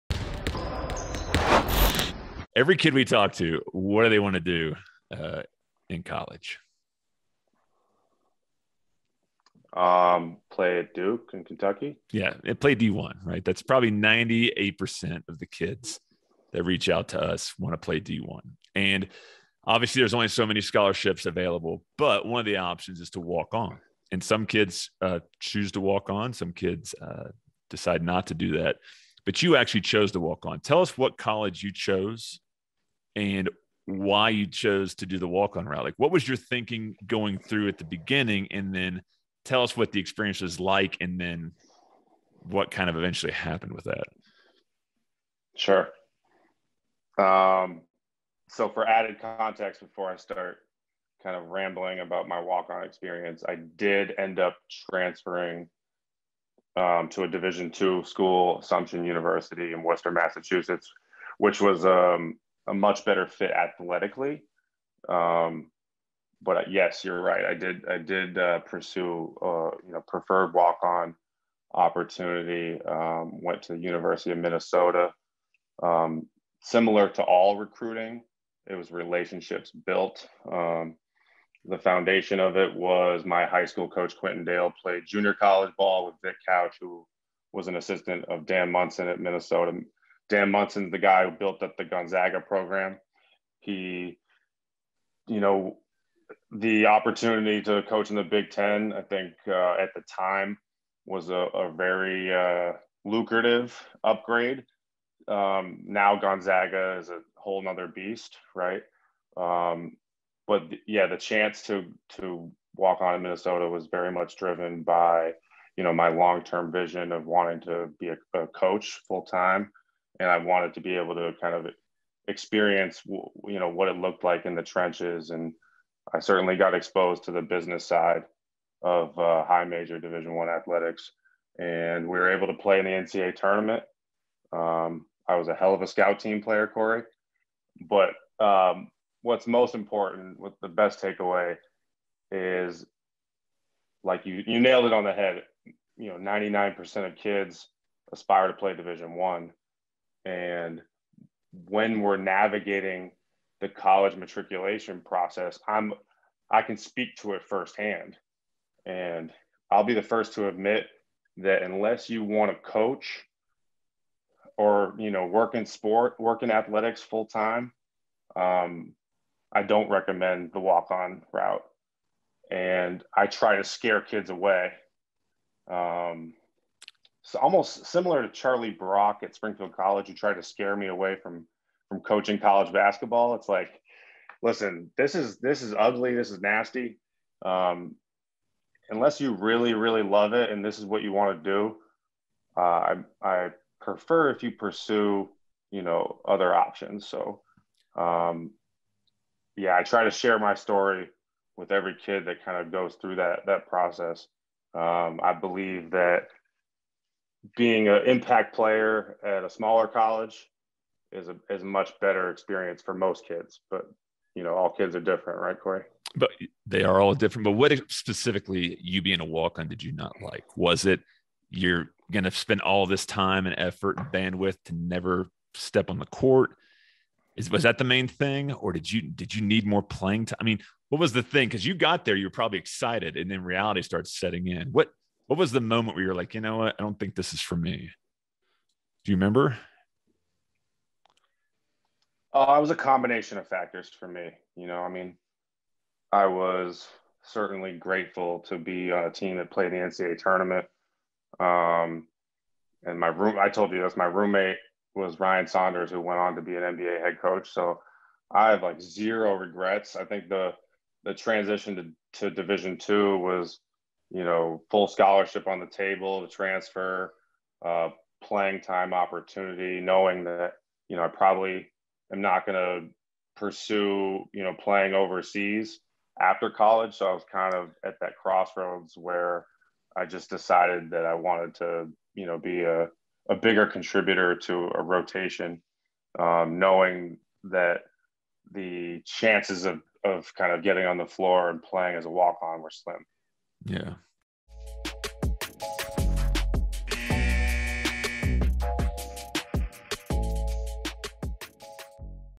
The Pros + Cons of Being a Division 1 Basketball Walk On feat. IMG Academy's Coach Alex Popp
In this highlight clip, IMG Academy's Alex Popp talks about his experience as a walk on at the University of Minnesota.  He spent one year at the Big Ten school before transferring to Assumption University.  Alex says unless you want to work in college athletics or be a coach, he would not recommend this route.  He breaks down the factors that led him to transferring to the right fitting level.  We discuss more details about being a walk on in the full podcast.